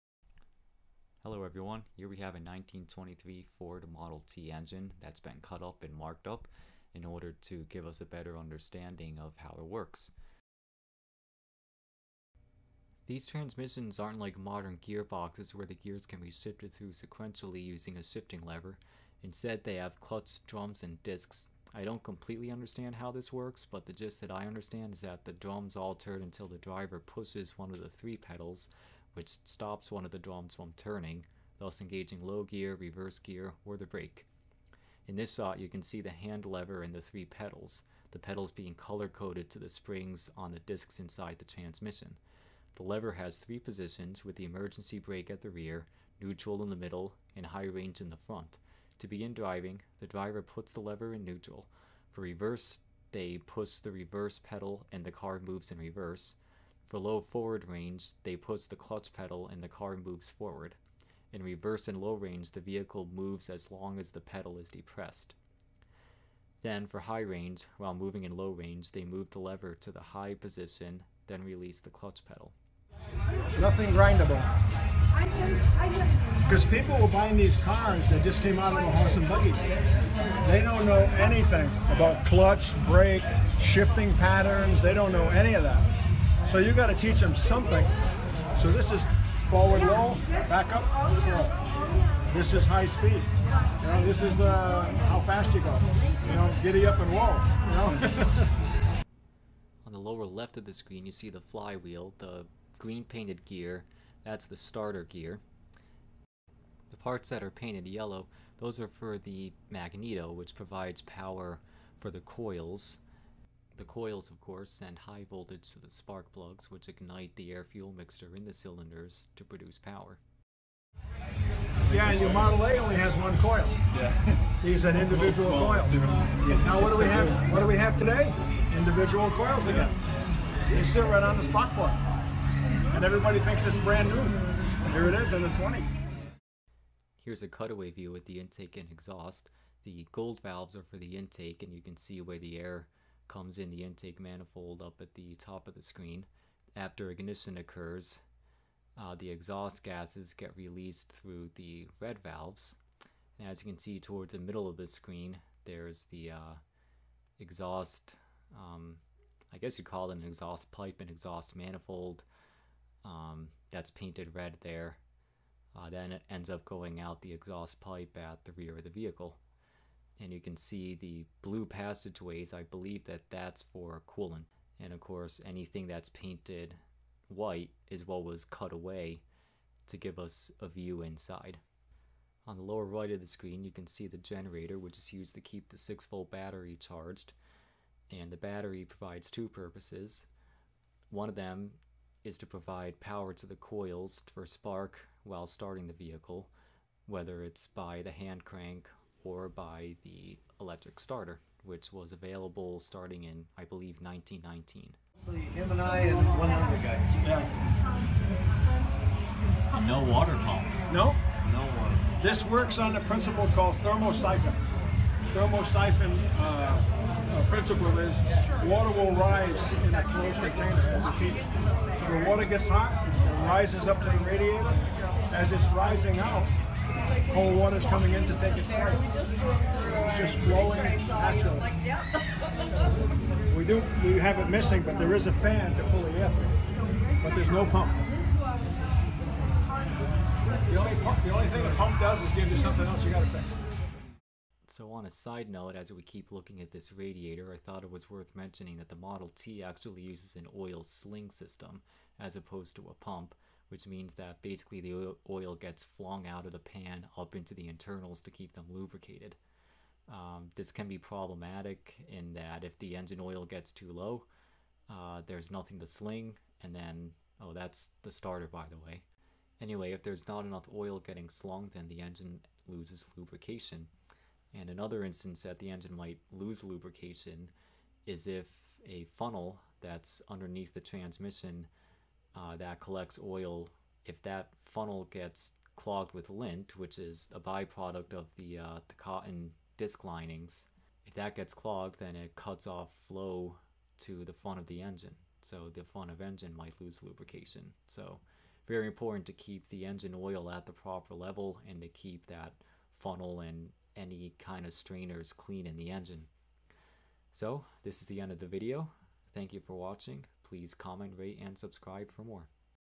Inside a 1923 Ford Model T engine (cutaway)
A cutaway of an actual 1923 Ford Model T engine.